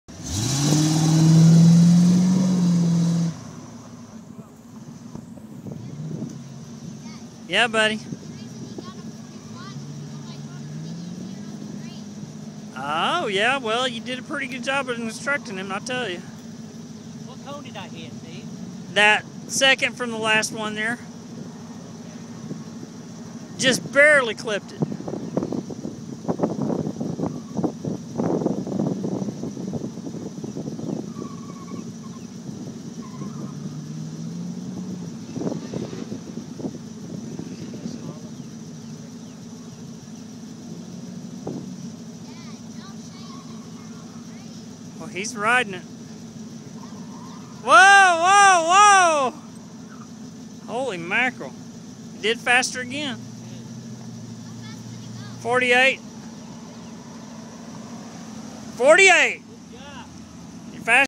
Shay Hoog does his first Autocross with Tulsa Vette Set
DescriptiShay Hoog does his first Autocross with Tulsa Vette Seton